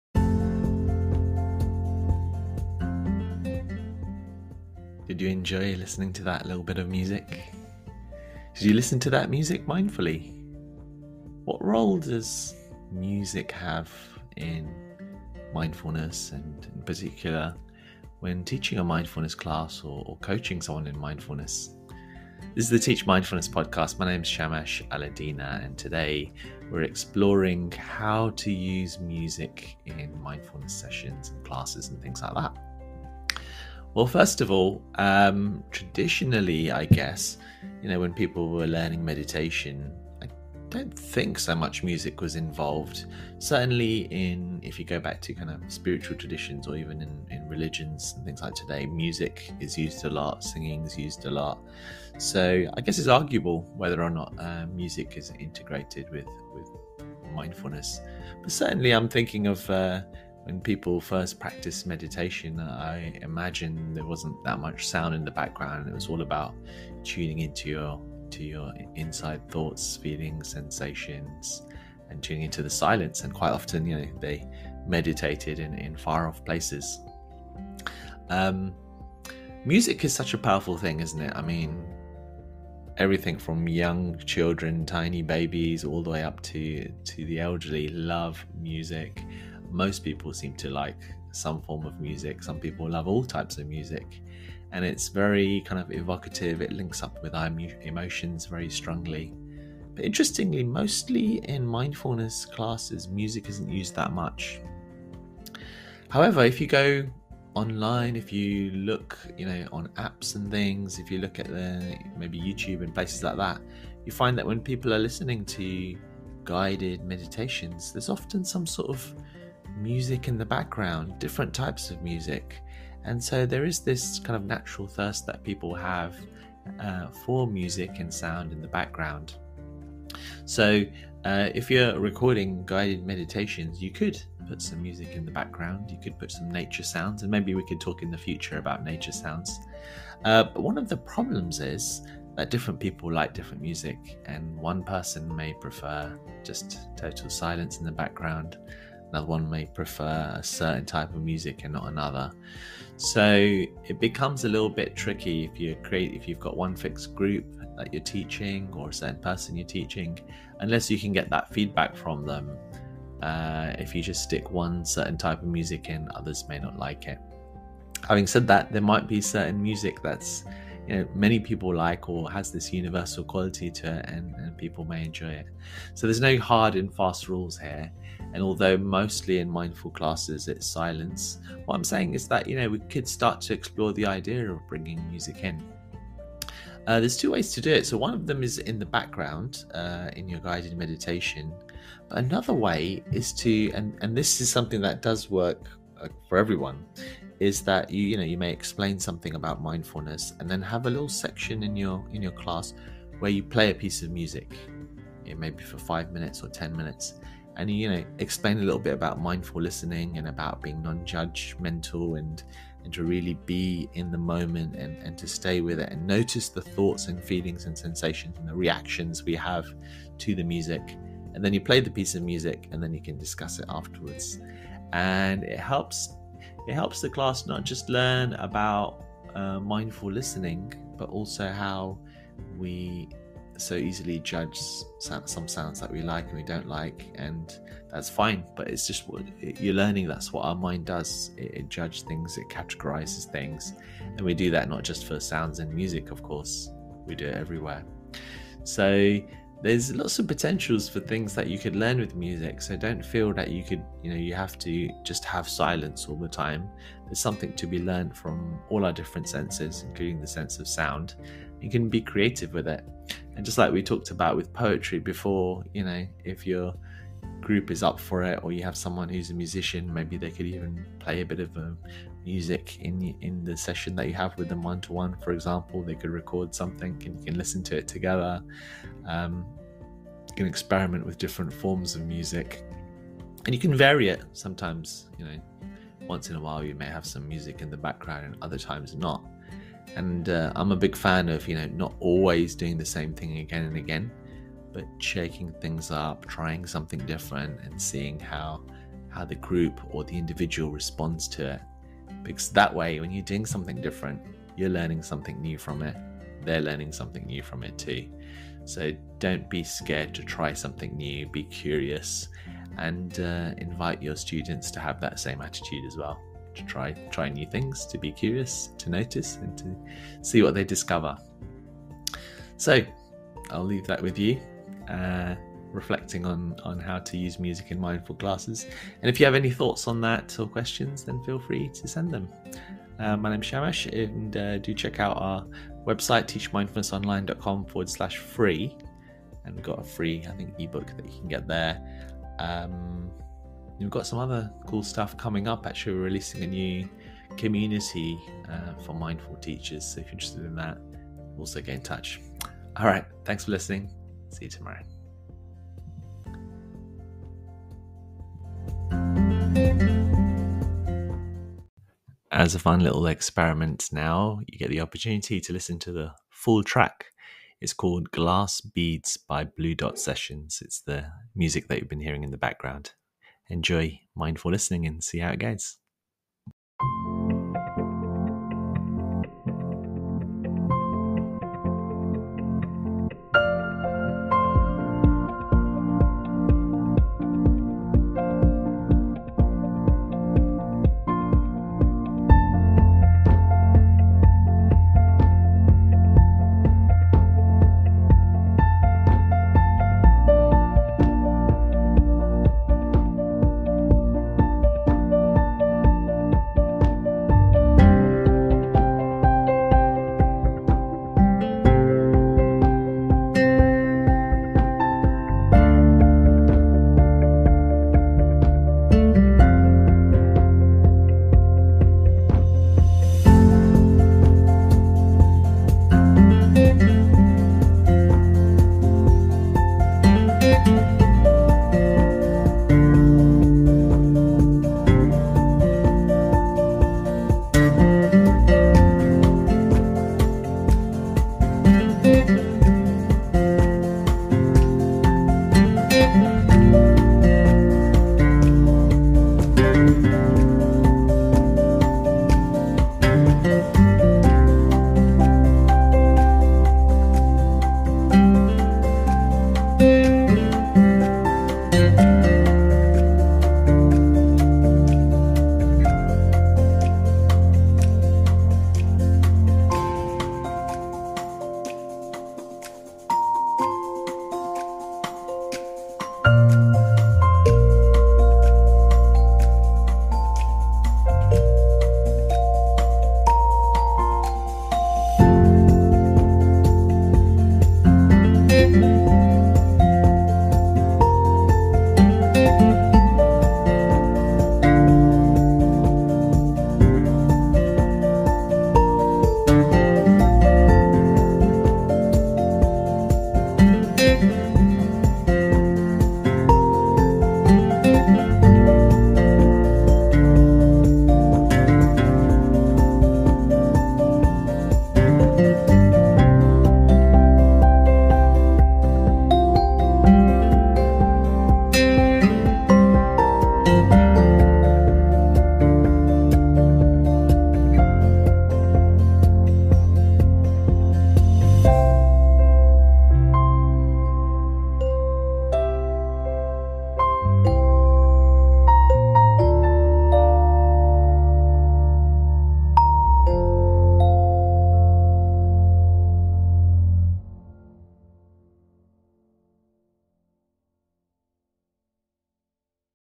#16 - Is Music Even Allowed in Mindfulness Classes? | Teach Mindfulness Podcast
What to Teach Mindfulness?